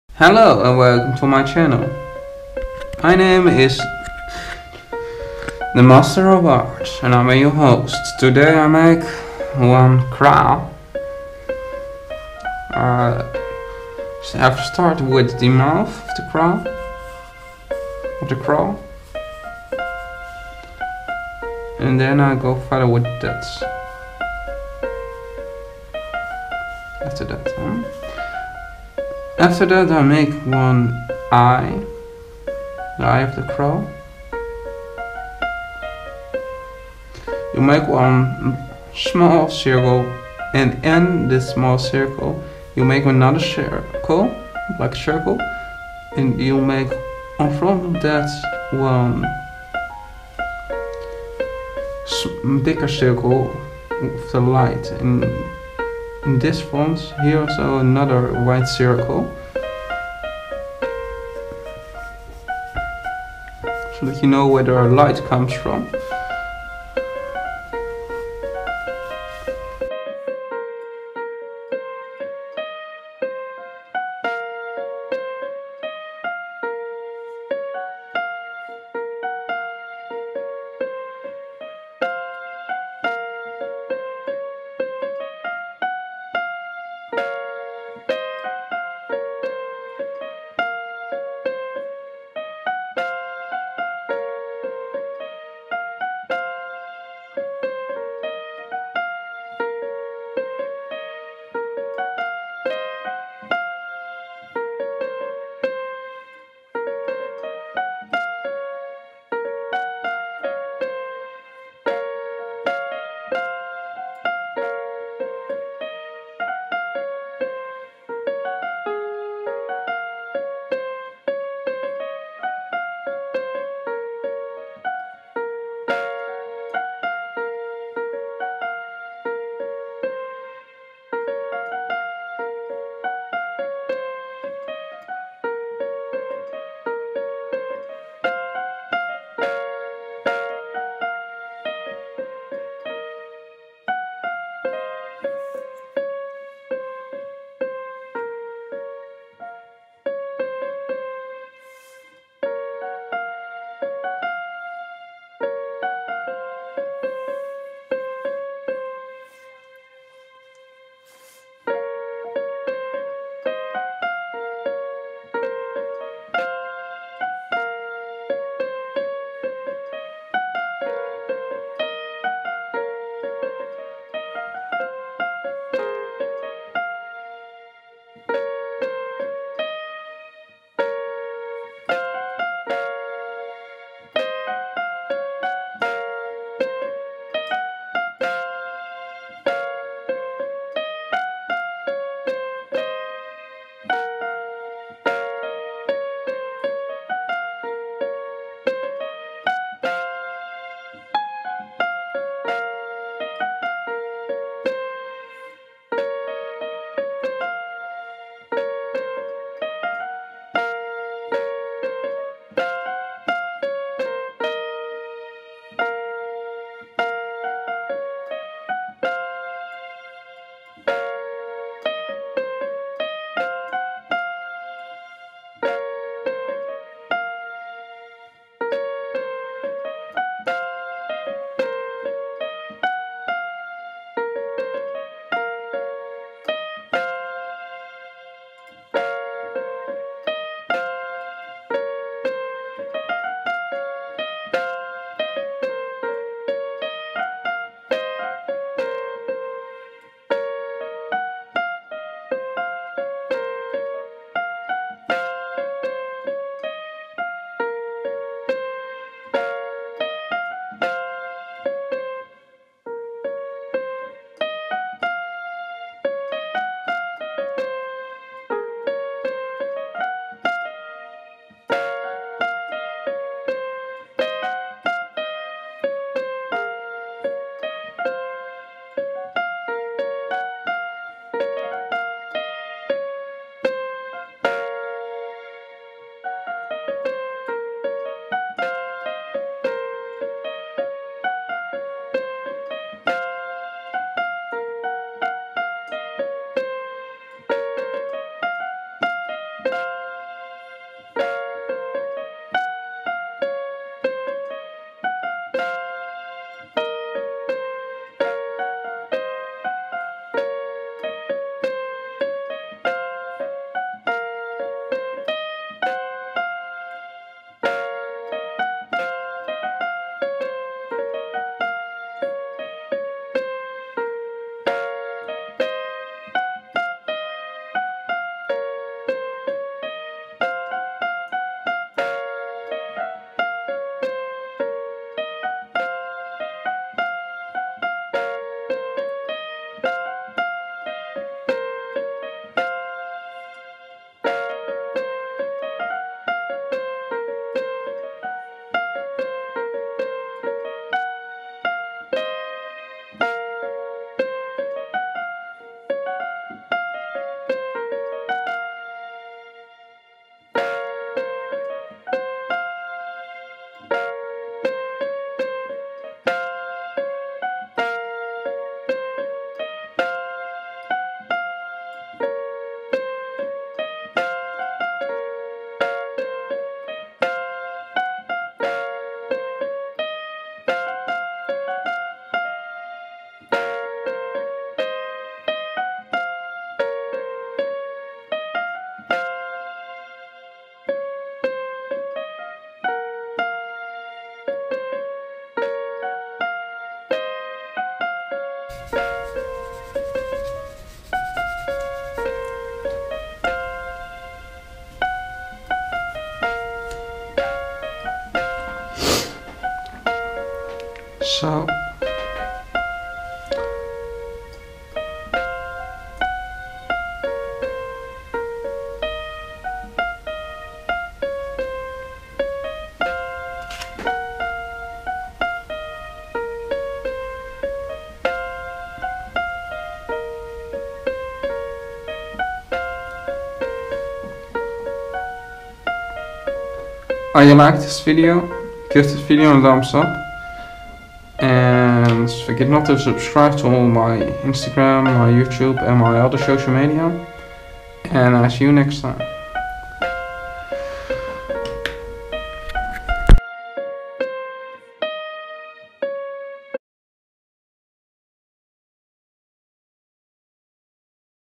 how to draw a Crow.' {master of arts}
to day i want to draw a crow. for you i hop you like the weal that i how this.

The mirror of the true is my all round vegan animal rights active channel + philosophy and psychology channel.'

google plus